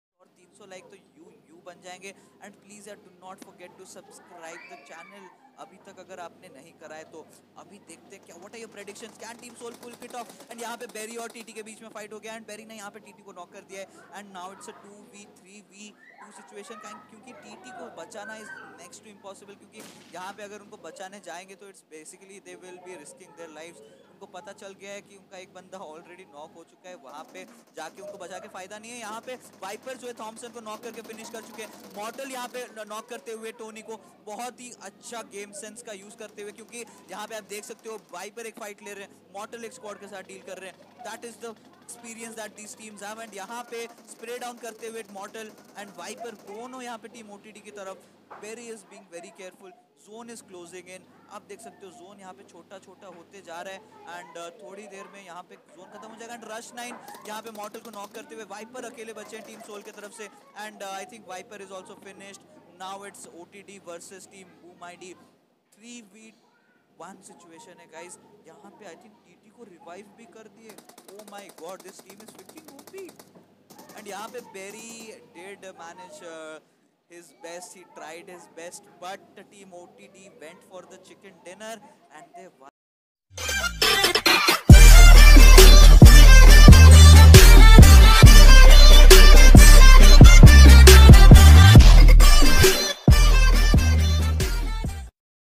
SOUL VS OTD VS BOOM ID | LAST ZONE FIGHT | WHO GOES FOR THE CHICKEN
This is our come back video .Plz support .